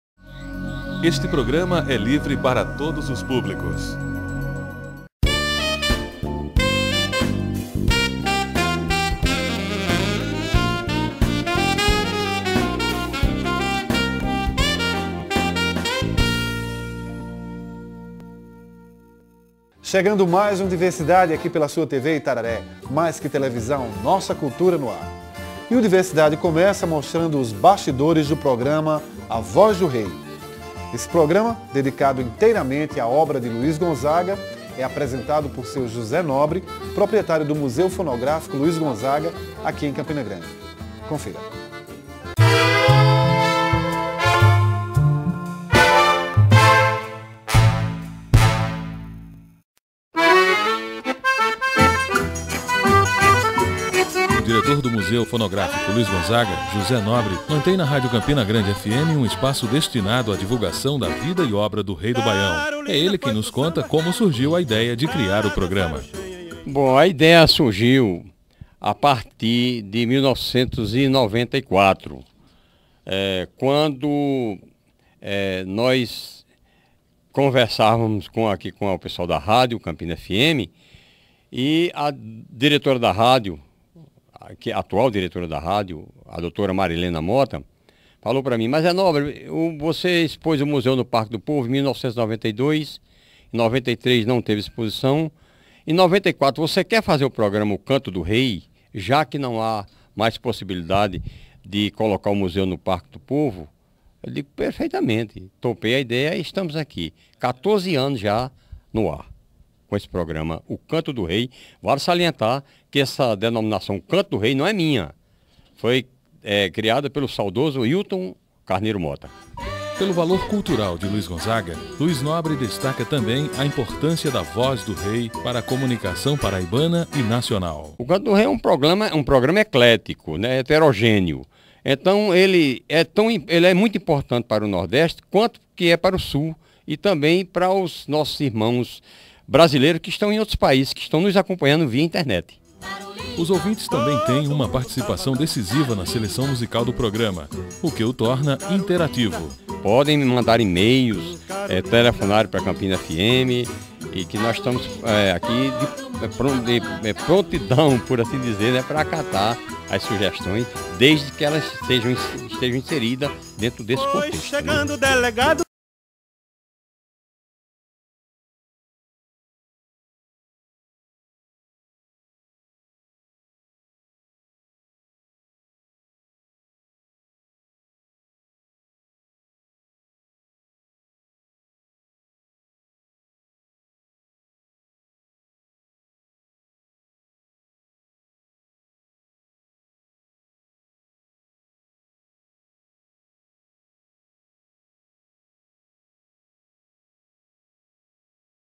A Voz do Rei
Bastidores do programa com José Nobre sobre a vida e obra do rei do baião, Luiz Gonzaga.